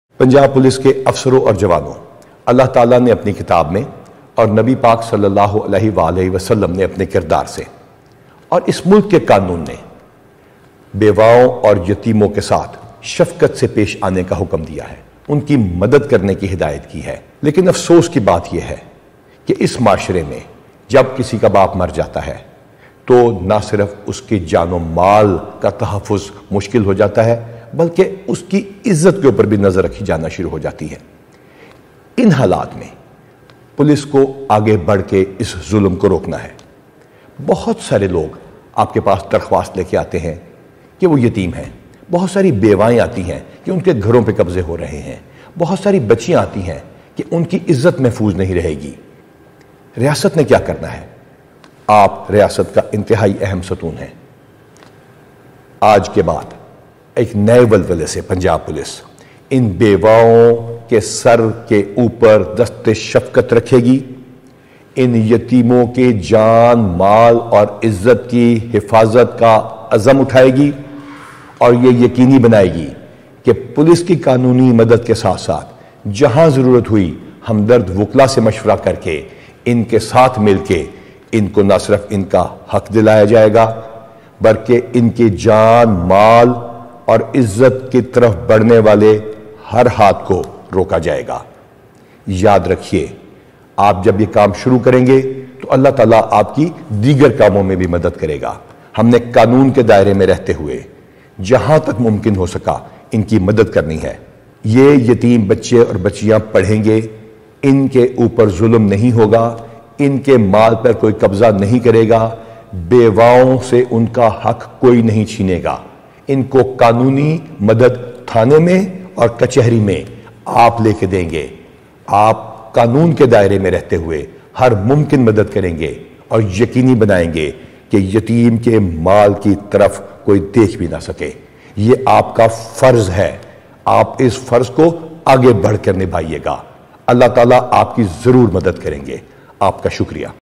Punjab Police will provide free assistance to the orphans and widows in courts to get their rights.
یتیموں اور بیواوں کو  ان کا حق دلوانے کے لیے پنجاب پولیس تھانوں کے ساتھ ساتھ کچہری میں بھی ہر ممکن  مدد فراہم کرے گی۔
آئی جی پنجاب ڈاکٹر عثمان انور نے پولیس فورس کو مزید کیا اہم ہدایات دیں ، جانیں اس ویڈیو میں